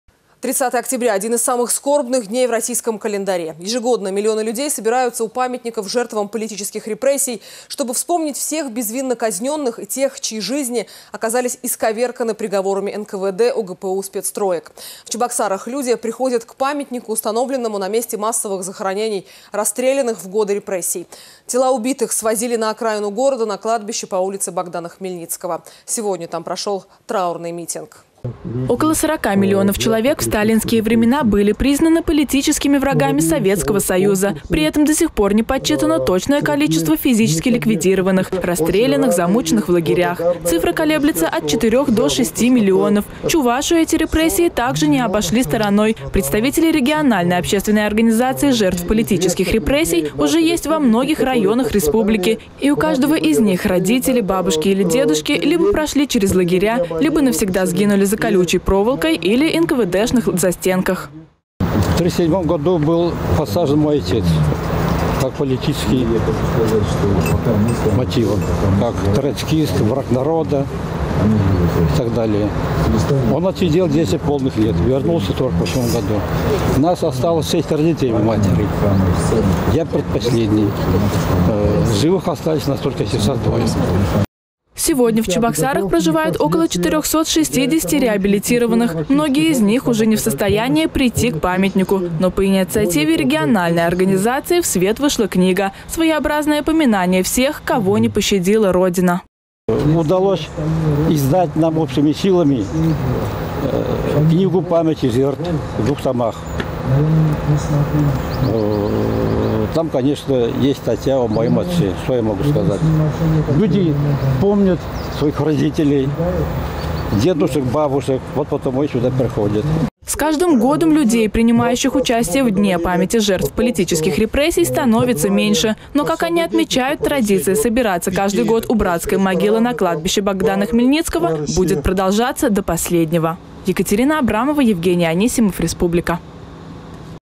В День памяти жертв политических репрессий в Чебоксарах прошел траурный митинг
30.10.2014
30 октября — один из самых скорбных дней в российско календаре. Ежегодно миллионы людей собираются у памятников жертвам политических репрессий, чтобы вспомнить всех безвинно казненных и тех, чьи жизни оказались исковерканы приговорами НКВД, ОГПУ, Спецтроек. В Чебоксарах люди приходят к памятнику, установленному на месте массовых захоронений расстрелянных в годы репрессий. Тела убитых свозили на окраину города, на кладбище по улице Богдана Хмельницкого. Сегодня там прошел траурный митинг.